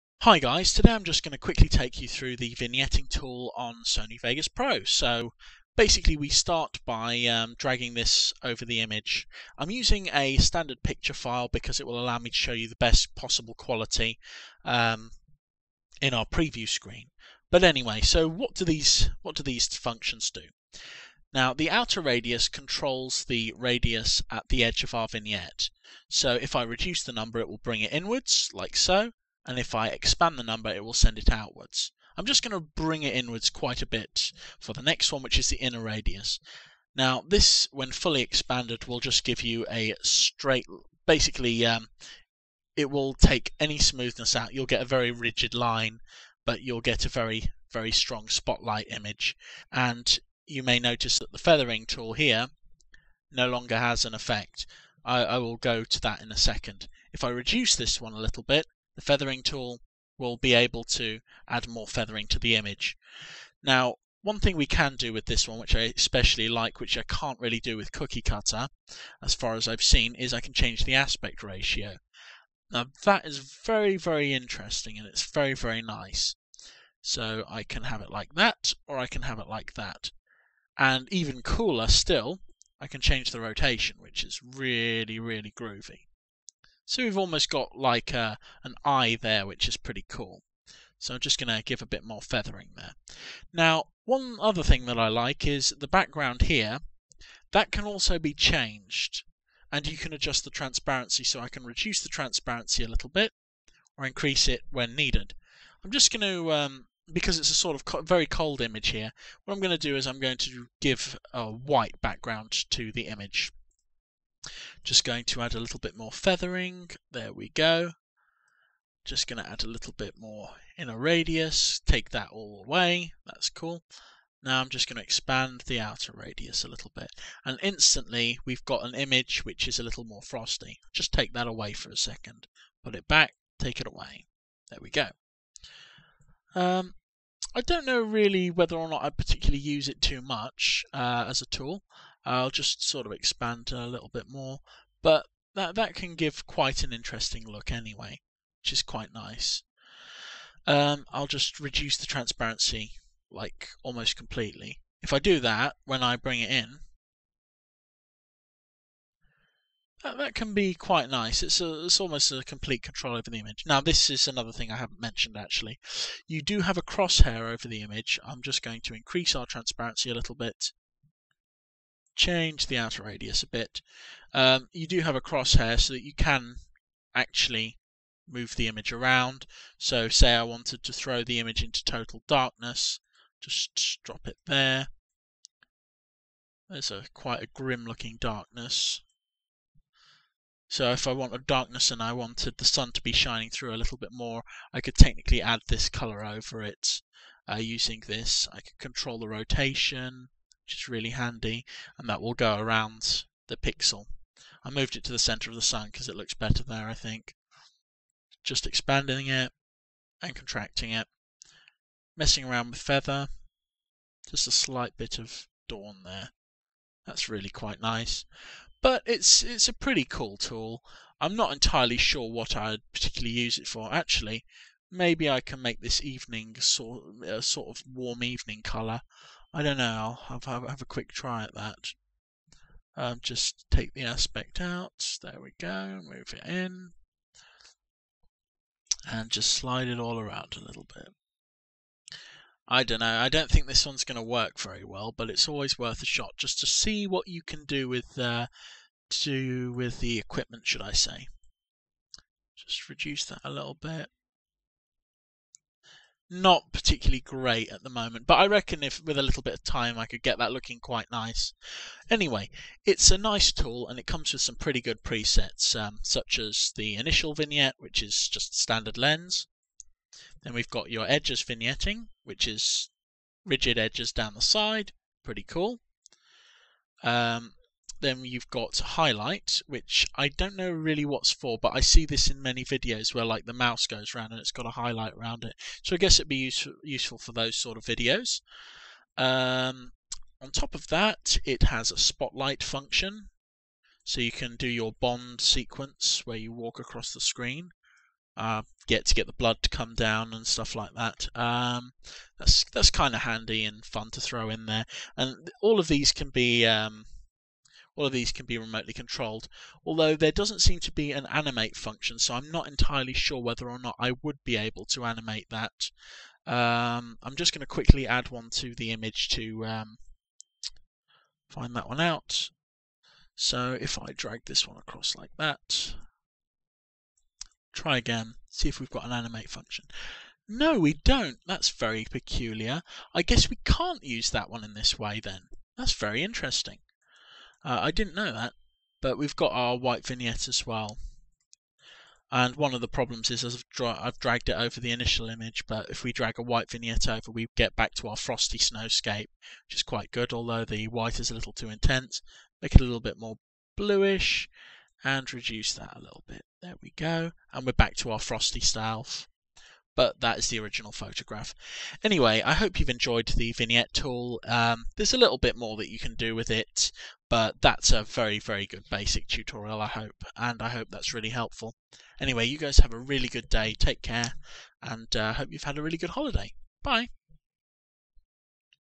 Vignette tool in Sony Vegas pro 14
A quick introduction to the vignette tool in Sony Vegas Pro 14